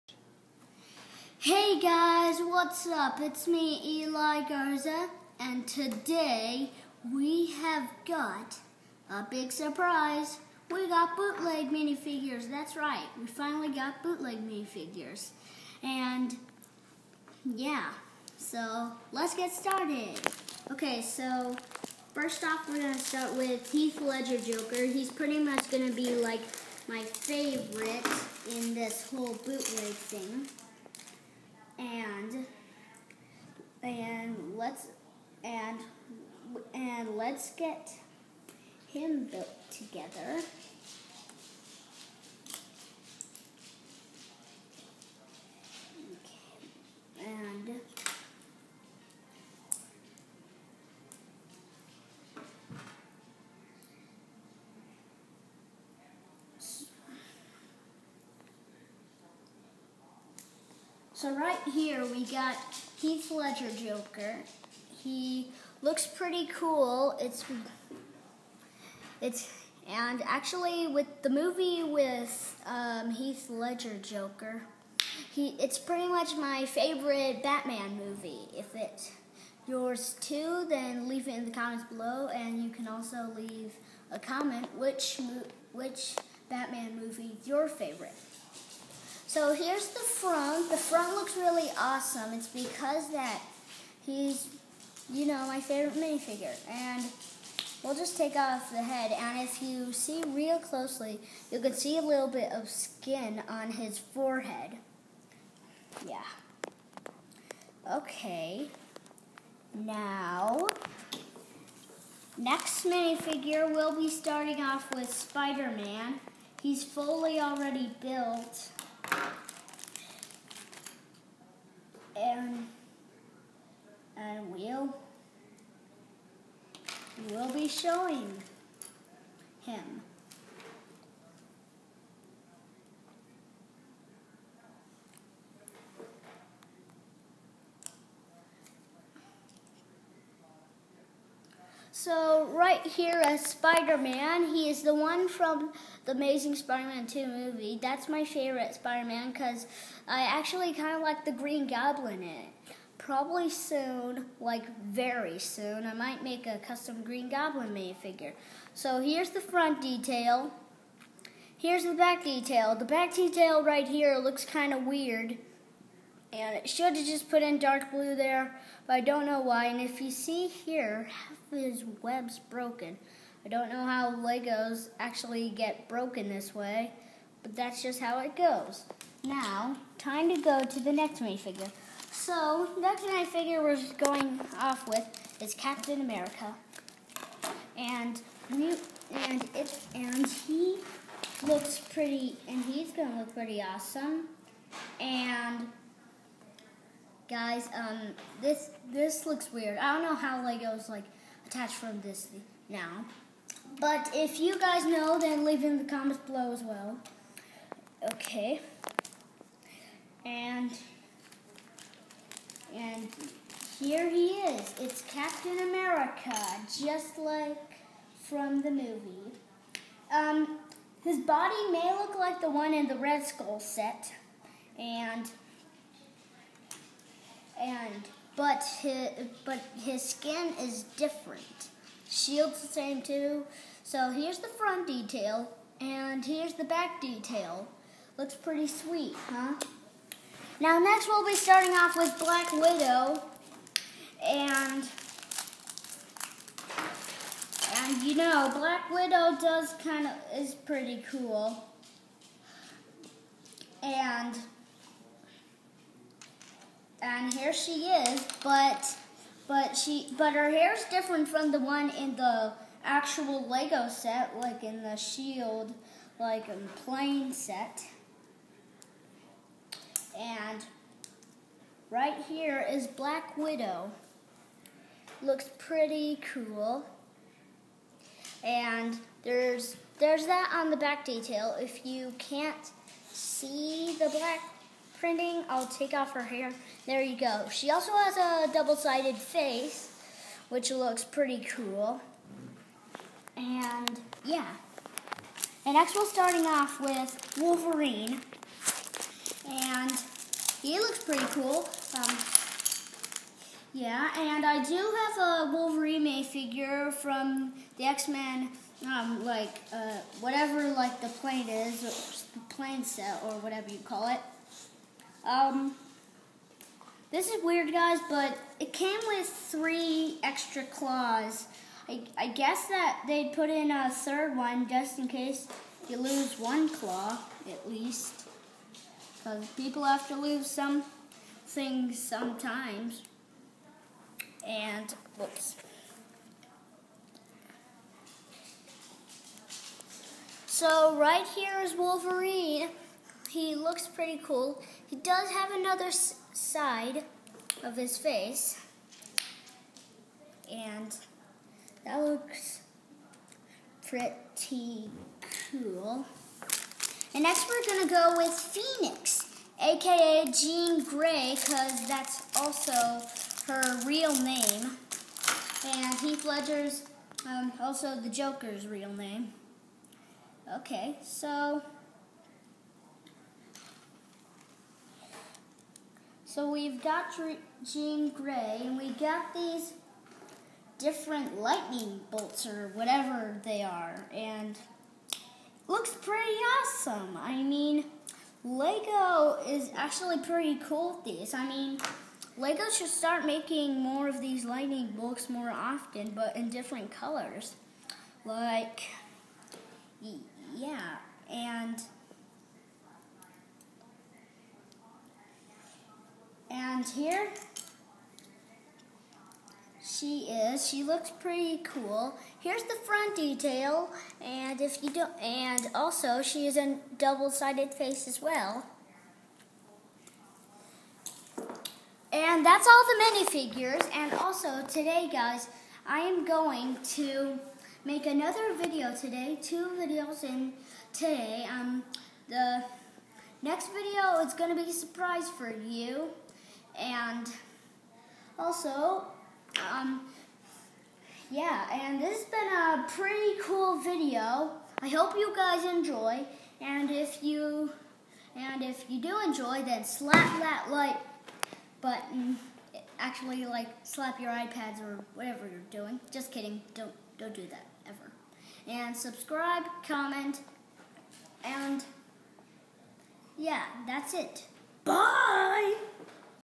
Lego bootleg minifigures review
Today I went to the post office and got my new minifigures!